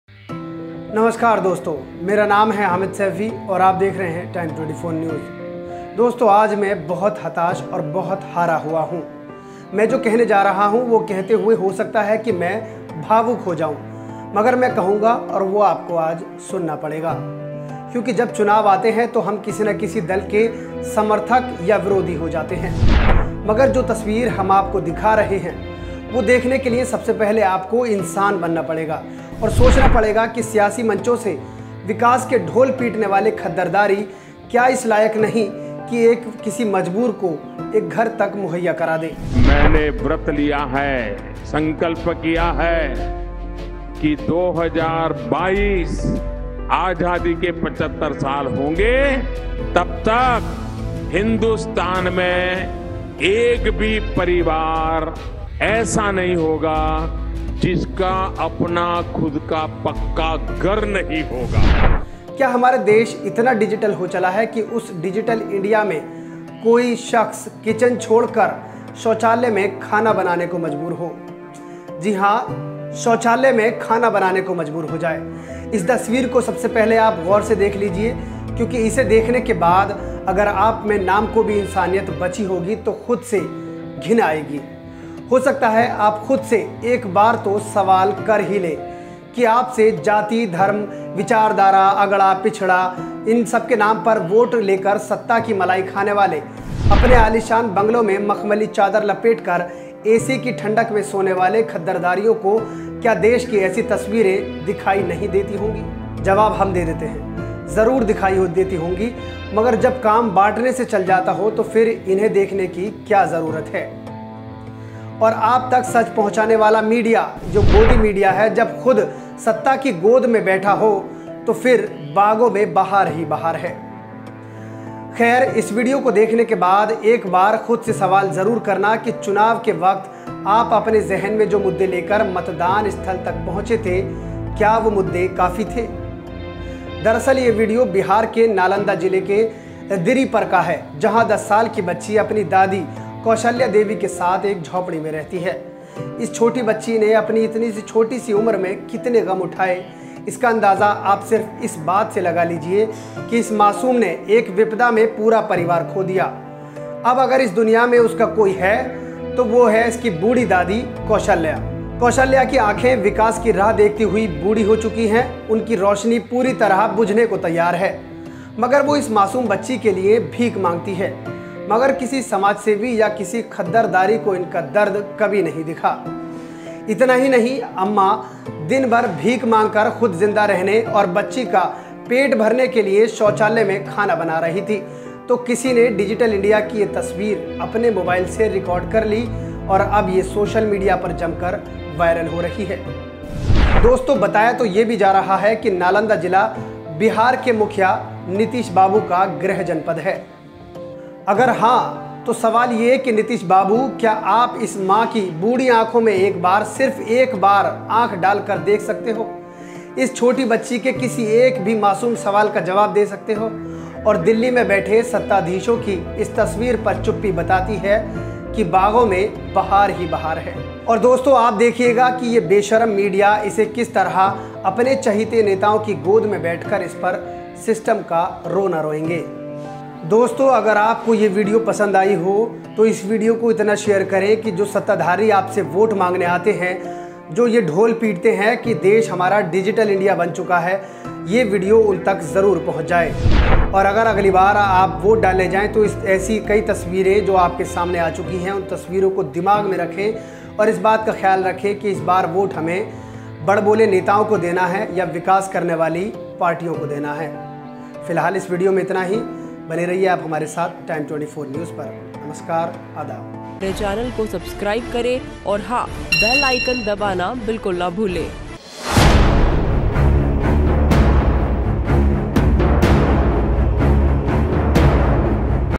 ये थी बूढ़ी दादी और पोती के टॉयलेट में रोटी बनाने की सच्चाई, रोंगटे खड़े कर देने वाली रिपोर्ट!
दोस्तों नामस्कर मैं हामिद सैफी और आज मैं बहुत हताश, बहुत हारा हुआ हूँ। मैं जो कहने जा रहा हूँ वो कहते हुए हो सकता है मैं भावुक हो जाऊं, मगर मैं कहूंगा, और वो आपको भी सुनना पड़ेगा।
क्योंकि जब चुनाव आते हैं तो हम किसी ना किसी दल के समर्थक और विरोधी हो जाते हैं।
मगर जो तस्वीर हम आपको दिखा रहे हैं  वो देखने के लिए सबसे पहले इंसान बनना पड़ेगा।
और सोचना पड़ेगा कि सियासी मंचो से विकास के ढोल पीटने वाले खद्दरधारी, क्या इस लायक नहीं कि वो किसी मजबूर हो एक घर मुहैया करा दें? क्या डिजिटल हो चला है कि उस डिजिटल इंडिया में कोई किचन छोड़कर कर शौचालय में खाना बनाने के लिए मजबूर हो जाये ?
इस तस्वीर को ज़रा गौर से देख लीजिए क्योंकि इसे देखने के बाद अगर आप में नाम को भी इंसानियत बची होगी तो खुद से घिन आएगी हो सकता आप खुद से एक बार तो सवाल कर ही लें कि आप से जाति, धर्म, विचारधारा, अगड़ा, पिछड़ा के नाम पर वोट लेकर सत्ता की मलाई खाने वाले, अपने आलीशान बंगलों में मखमली चादर लपेट कर ऐसी की ढंड में सोने वाले खद्दरधारीयों को क्या देश की ऐसी तस्वीर दिखाई नहीं देती होंगी?
जवाब हम देते हैं ज़रूर दिखाई देती होंगी मगर जब काम बांटने से चल जाता हो तो फिर इन्हें देखने की ज़रूरत क्या है।
और आप तक सच पहुंचाने वाला मीडिया जब खुद सत्ता की गोद मे बैठा हो तो फिर तो बागों में बाहर है।
खैर इस वीडियो को देखने के बाद एक बार खुद से सवाल ज़रूर करना कि चुनाव के वक्त आप अपने ज़हन में जो मुद्दे लेकर मतदान स्थल पहुंचे थे वो काफी थे?
दरअसल ये वीडियो बिहार के नालंदा  ज़िले के दिरीपर का है। जहां 10 साल की बच्ची अपनी दादी कौशल्या देवी के साथ एक झोपड़ी में रहती है.
इस छोटी बच्ची ने अपनी इतनी छोटी सी उम्र में  कितने गम उठाये इसका अंदाज़ा आप सिर्फ इसी बात से लगा लीजिए कि इस मासूम ने एक विपदा में पूरा परिवार को दिया अब अगर इस दुनिया मे उसका कोई है तो वो हैं इसकी बूढ़ी दादी कौशल्या।
कौशल्या की आंखे विकास की राह देखती हुई बूढ़ी हो चुकी हैं, उनकी रौशनी पूरी तरह बुझने को तैयार है मगर वो इस मासूम बच्ची के लिए भीख मांगती हैं मगर समाजसेवी किसी खद्दरधारी को इनका दर्द नहीं दिखा। इतना ही नहीं अम्मा दिन भर भीख मांग कर जब अम्मा खुद ज़िन्दा रहने और बच्ची का पेट भरने के शौचलय में खाना बना रही थी।
तो किसी ने डिजिटल इंडिया की ये तस्वीर अपने मोबाइल से रिकॉर्ड कर लिया और अब इस वीडियो को सोशल मीडिया पर जमकर वायरल किया जा रहा है।
दोस्तो बताया तो यह भी जा रहा है कि नालंदा ज़िला बिहार के मुखिया नीतीश बाबू का गृहजनपद है।
अगर हाँ तो सवाल यह है कि नीतीश बाबू क्या आप इस मां की बूढ़ी आंखों में एक बार, सिर्फ एक आंख डालकर देख सकते हो। इस छोटी बच्ची किसी एक भी मासूम सवाल का जवाब दे सकते हो? और दिल्ली में बैठे सत्ताधीशों की इस तस्वीर पर चुप्पी बताती है कि बागों में बाहर है। और आप देखिएगा कि बेशर्म मीडिया इसे किस तरह अपने चहेते नेताओं की गोद में बैठकर इसपर  सिस्टम का रोना रोएंगे।